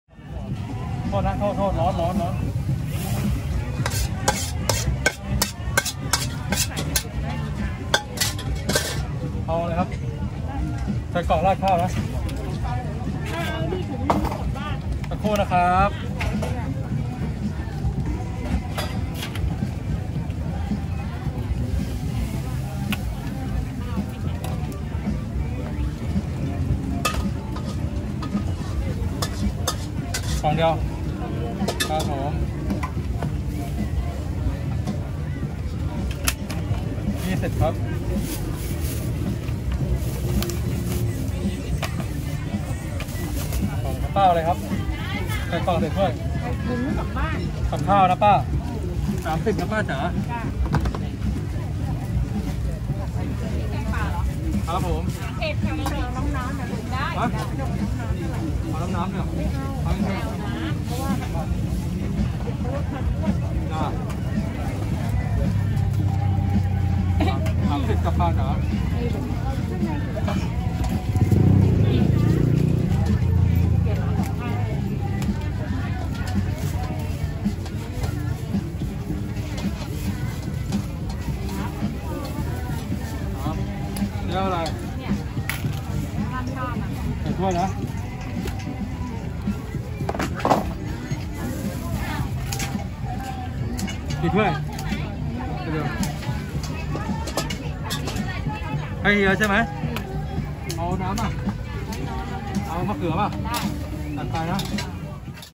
Sample ข้าวแกง 20 บาท!!!
ข้าวแกง20บาท ผัดใหม่ๆ ร้อนๆ พิกัด ตลาดนัดศาลารวมใจชน

เดินสายถ่ายคลิป ร้านยำ ร้านอาหารข้างทาง ตลาดนัดฯ สนับสนุนคนทำมาหากิน ติดต่อได้ที่ เพจ เดินสายถ่ายคลิป